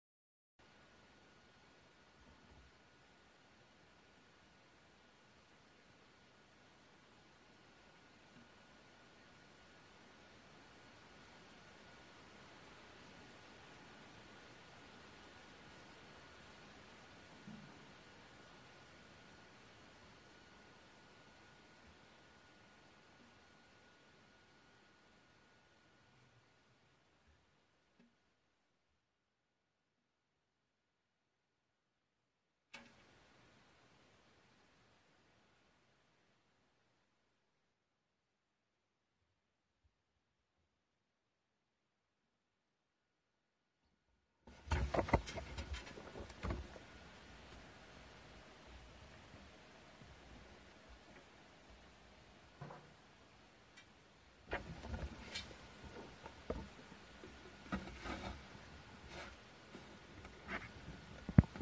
1-6-17 Traxxas Stampede 360fly 360 FPV Cockpit + Hud Test
Just testing, with a minor forward tilt.